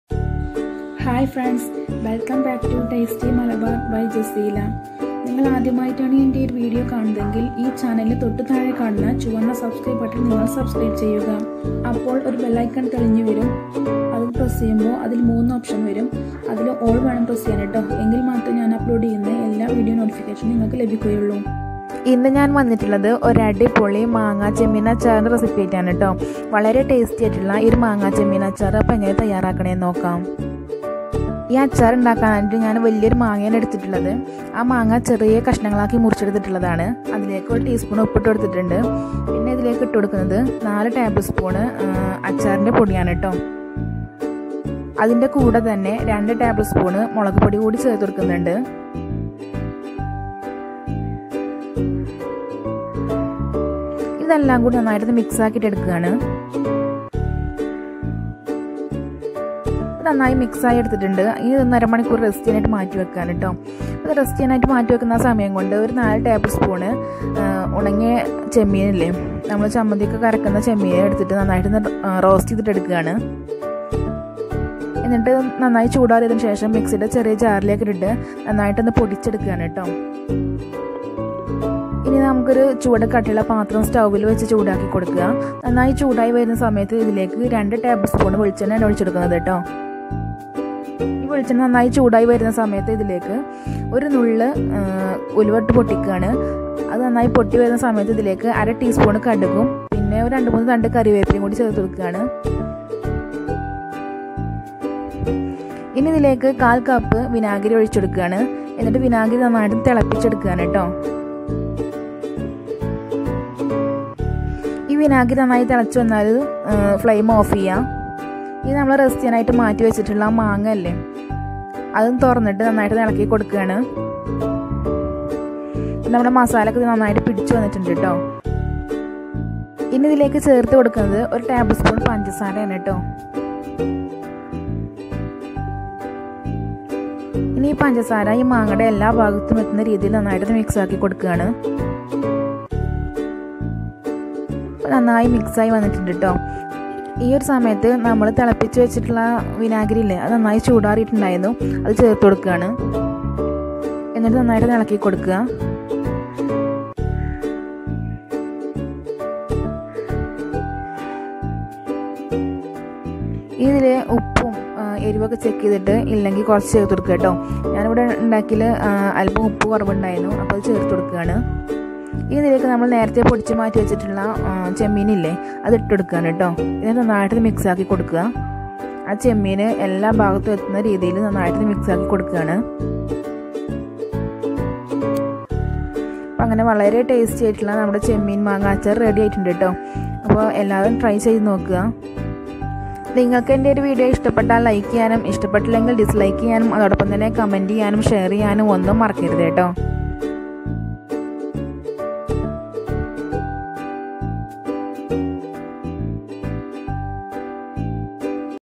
ഉണക്ക ചെമ്മീനും പച്ച മാങ്ങയും കൊണ്ട് കിടിലൻ അച്ചാർ|Dry Prawns & Mango Pickle|Tasty Malbar by jaseela
ചേരുവകൾ :

1. പച്ച മാങ്ങ 1 എണ്ണം
2. അച്ചാർ പൊടി 4 tblsp
3. മുളക് പൊടി 2 tblsp
4. ഉപ്പ് ആവശ്യത്തിന്
5. പഞ്ചസാര 1 tblsp
6. ഉലുവ 1 നുള്ള്
7. വെളിച്ചെണ്ണ 2 tblsp
8. കടുക് 1/2 tsp
9. കറിവേപ്പില 3 തണ്ട്
10. വിനാഗിരി 1/4 കപ്പ്‌